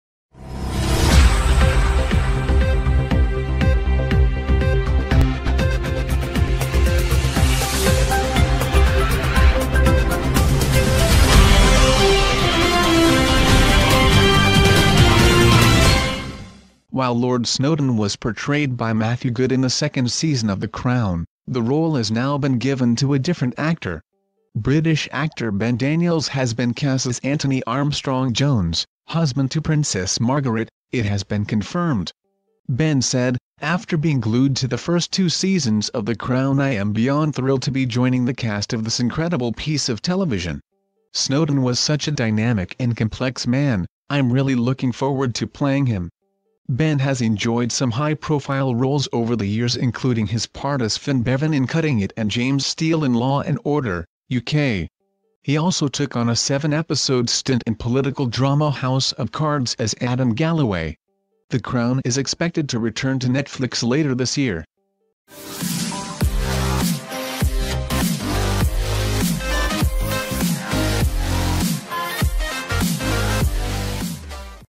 The Crown season 3 cast: House of Cards star REVEALED as Lord Snowdon replacement
While Lord Snowdon was portrayed by Matthew Goode in the second season of The Crown, the role has now been given to a different actor.
British actor Ben Daniels has been cast as Antony Armstrong-Jone